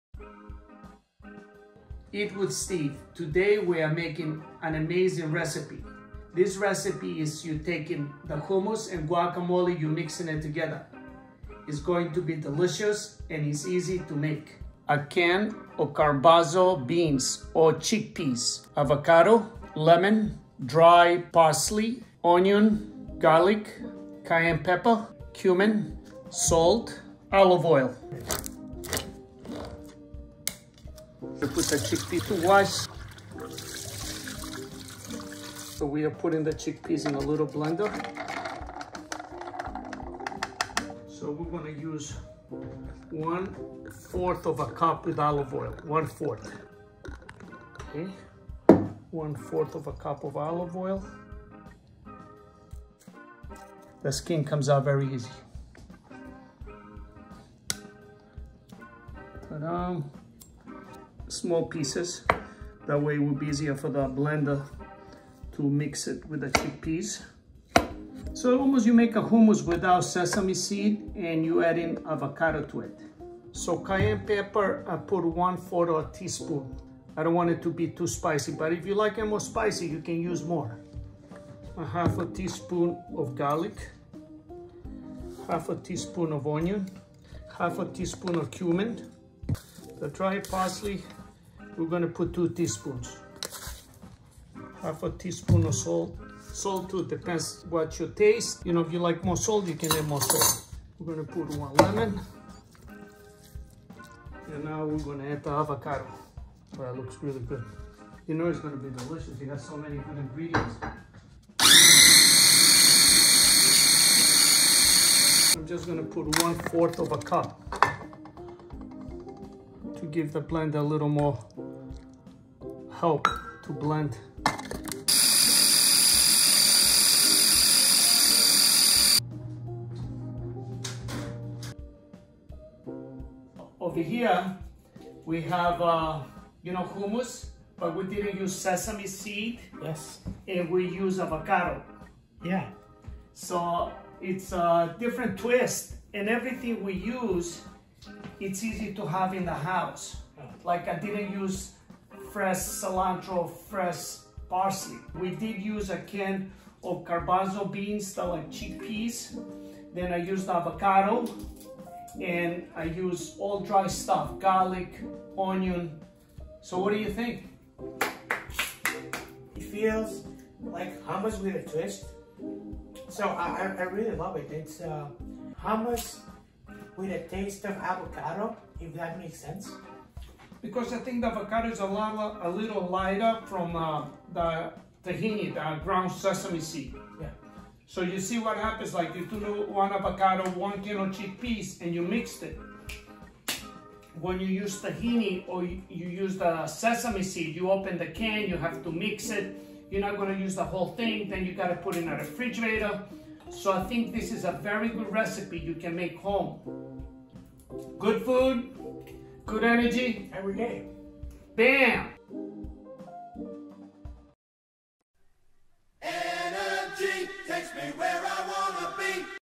Chickpea with avocado spread
Chickpea &amp; Avocado Hummus

Ingredients
1 can chickpeas
1 avocado
1 lemon
dried parsley flakes
Onion powder
garlic powder
Cayenne pepper
cumin
Salt
olive oil

Directions
Wash chickpeas in a strainer and place in a food processor
Add ¼ cup olive oil
¼ tsp cayenne pepper (or to liking)
½ tsp garlic powder
½ tsp onion powder
½ tsp cumin
2 tsp parsley flakes
½ tsp salt (or to liking)
Juice of 1 lemon
Peel, chop and add avocado to chickpeas
Add ¼ cup water and blend until smooth